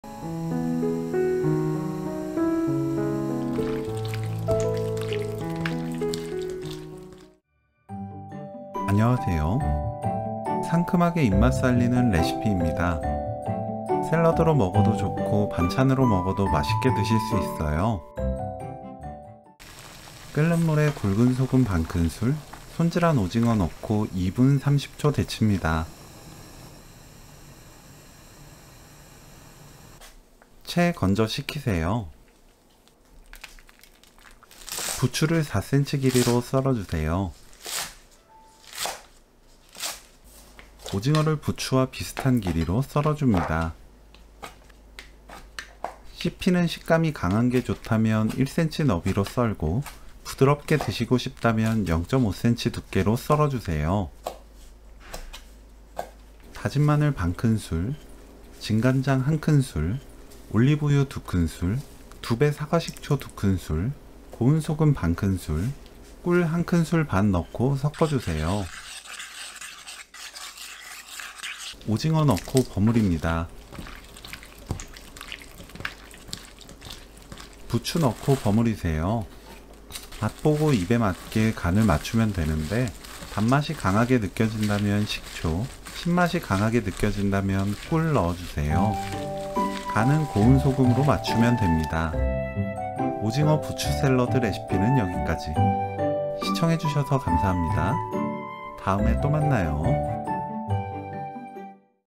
지금부터 부추무침에 '올리브유' 넣으세요! 부추를 바닥까지 싹싹 긁어먹은건 처음입니다. 💯 평생 써먹는 부추 요리 만드는법.🥇 부추오징어샐러드 맛있게 만드는법
🔔 부추오징어샐러드. (1T=밥숟가락)
부추 70g, 오징어 (중) 2마리, 굵은소금 ½T, 다진 마늘 ½T, 진간장 1T, 올리브유 2T, 2배사과식초 2T, 고운소금 ½T (3g), 꿀 1½T.

✨ 부추오징어샐러드. (자세한 레시피는 영상을 참고하세요.)
1. 끓는 물에 굵은소금 1/2T, 손질한 오징어 넣고 2분 30초 익힌다.
2. 부추, 오징어 썬다.
3. 다진 마늘 ½T, 진간장 1T, 올리브유 2T, 2배사과식초 2T, 고운소금 ½T (3g), 꿀 1½T 섞는다.
4. 오징어 넣고 버무린다.
5. 부추 넣고 버무린다.